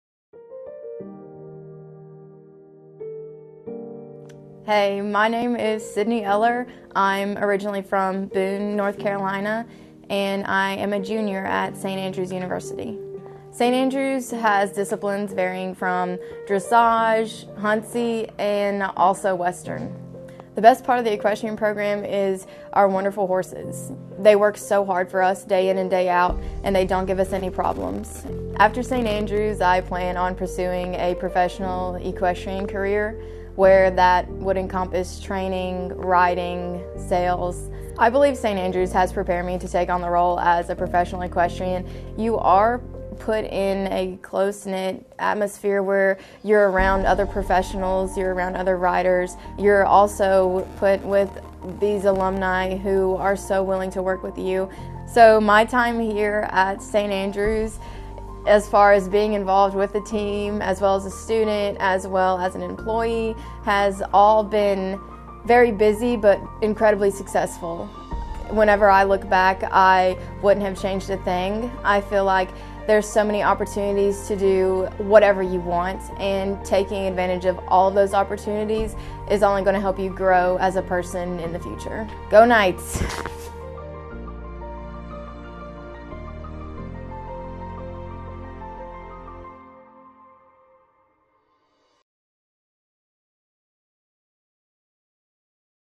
Learn more about our Equestrian Program at St. Andrews! We are celebrating 50 years.
Have an interest in Riding?   Check out the video of Sydney Eller, a current student at St. Andrews University, a branch of Webber International University.   Learn about programs related to Equestrian and about her experience riding.   St. Andrews University is celebrating it's 50th anniversary of Equestrian at the University.